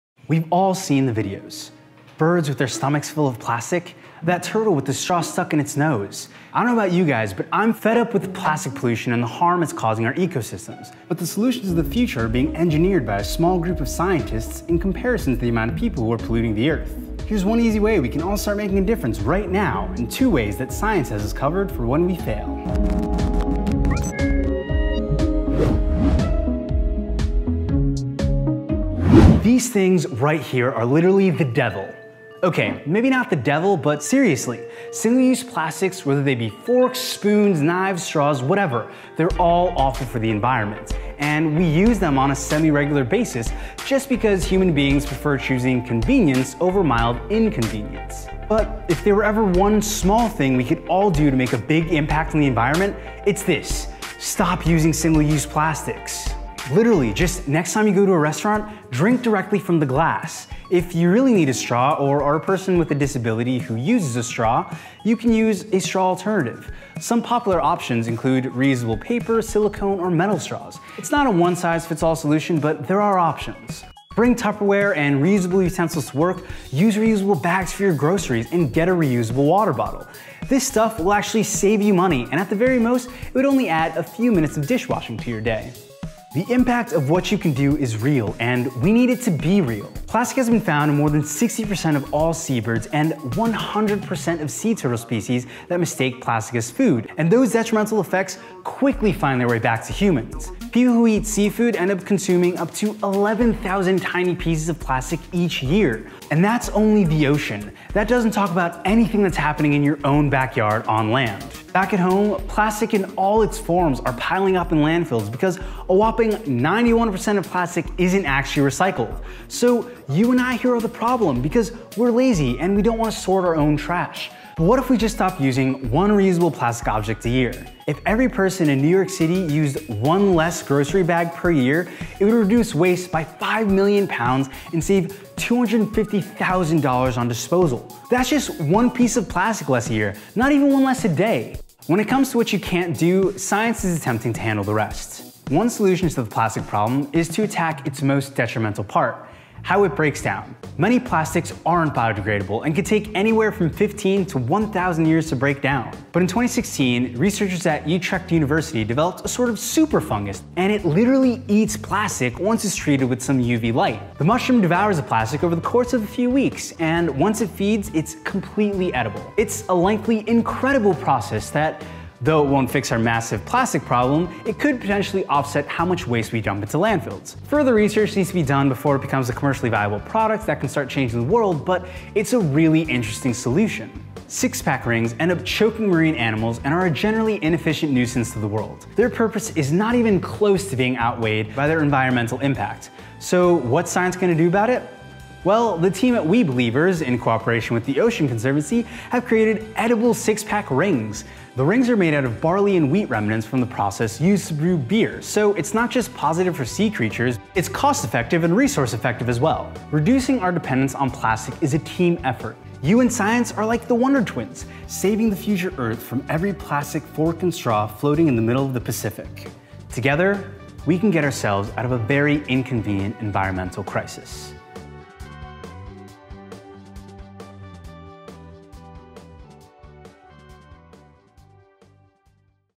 مشكلة التلوث البلاستيكي
يهدّد التلوث البلاستيكي في محيطاتنا الحياة البشرية والبحرية معاً. فكيف يُمكننا التغلّب على هذه الأزمة؟ وما هي الحلول العلمية المقترحة؟